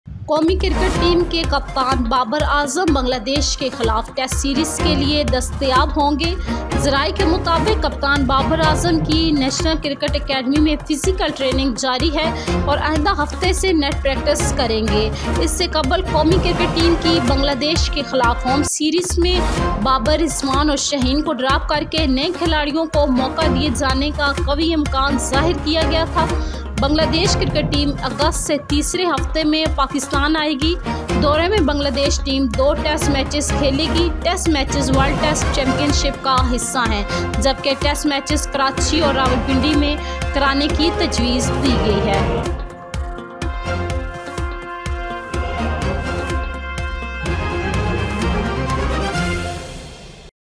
Captain Babar Azam will be available for the Test series against Bangladesh
National team captain Babar Azam will be available for the Test series against Bangladesh. According to sources, captain Babar Azam's physical training is going on at the National Cricket Academy and he will do net practice from next week.
Earlier, there was a strong possibility of dropping Babar, Rizwan and Shaheen in the national cricket team's home series against Bangladesh and giving a chance to new players.